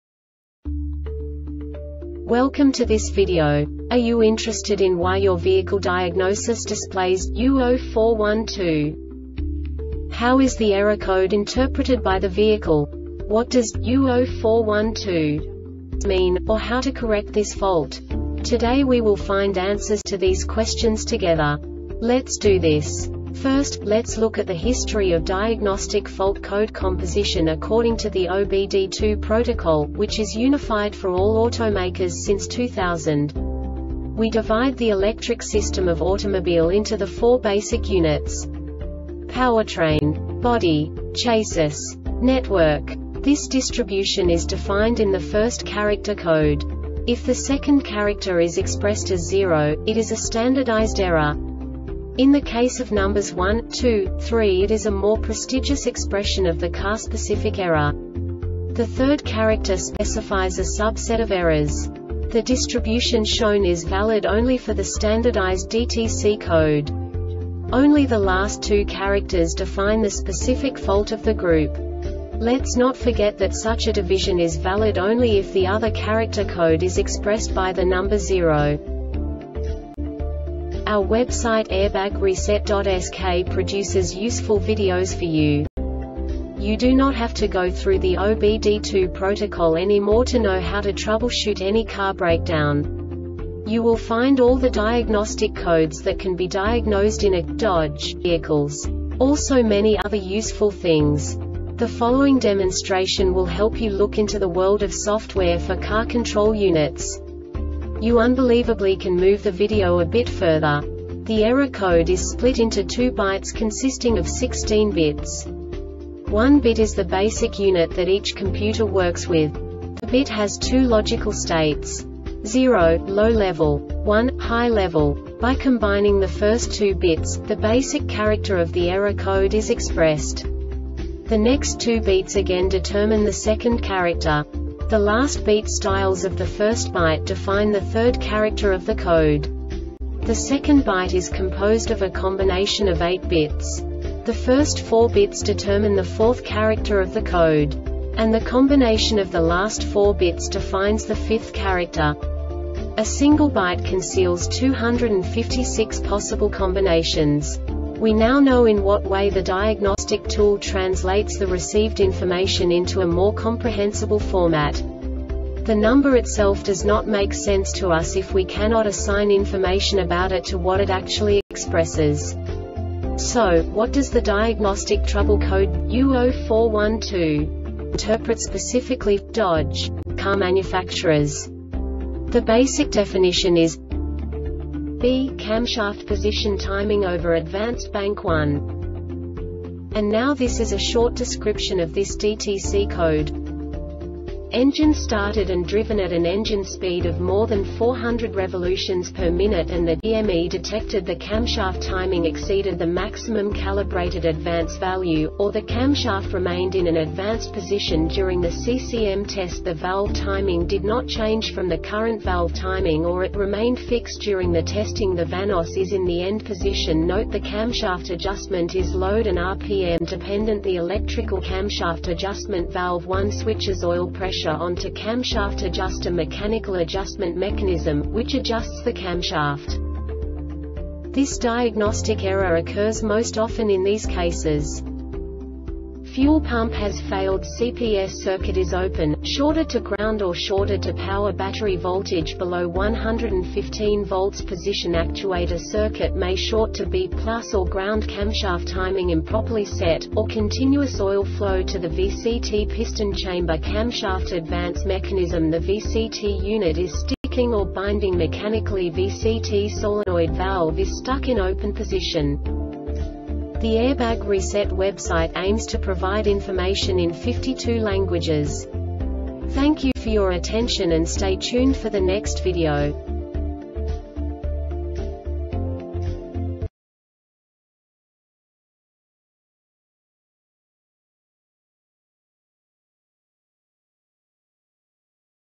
DTC Dodge U0412 Short Explanation
Contents:
0:21 Basic DTC analysis according to OBD2 protocol standard.
1:48 Insight into programming
2:58 A diagnostically understandable description of the Diagnostic Trouble Code
3:05 Basic error definition

Make:
Dodge
Code:
U0412
Definition:
“B” Camshaft Position Timing Over-Advanced (Bank 1)
Description:
Engine started and driven at an engine speed of more than 400rpm;and the DME detected the camshaft timing exceeded the maximum calibrated advance value,or the camshaft remained in an advanced position during the CCM test.The valve timing did not change from the current valve timing or it remained fixed during the testing.The VANOS is in the end position.Note: The camshaft adjustment is load- and RPM dependant.The electrical camshaft adjustment valve 1 switches oil pressure onto camshaft adjuster (mechanical adjustment mechanism),which adjusts the camshaft.
Cause:
 Fuel pump has failed CPS circuit is open,shorted to ground or shorted to power Battery voltage below 11.5 volts Position actuator circuit may short to B+ or Ground Camshaft timing improperly set,or continuous oil flow to the VCT piston chamber Camshaft advance mechanism (the VCT unit) is sticking or binding mechanically VCT solenoid valve is stuck in open position